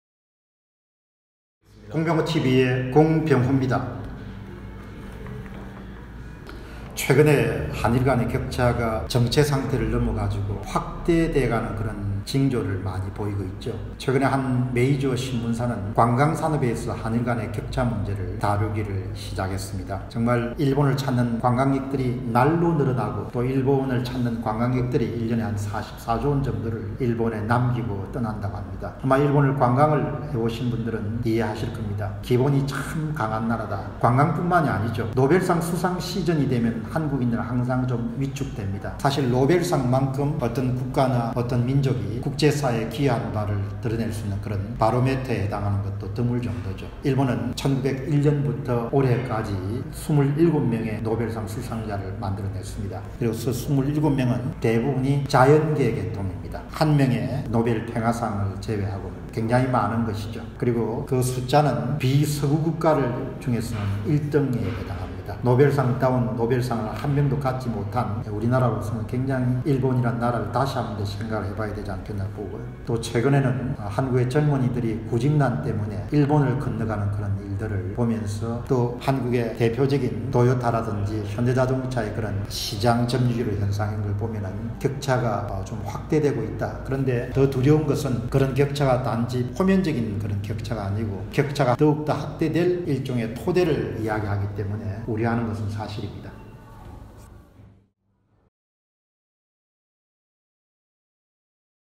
[한일 시리즈] 한일간의 격차가 다시 확대되는 것은 아닌가?
한일간의 격차가 다시 확대되는 것은 아닌지,
공병호TV가 깊이있게 전합니다🔍

✔ '한일의 미래'는 시리즈로 연이어 업로드 됩니다.
✔ 욕설, 비하 등 기본 매너에 어긋나지 않는 어떠한 질문, 반박, 토론 모두 환영합니다.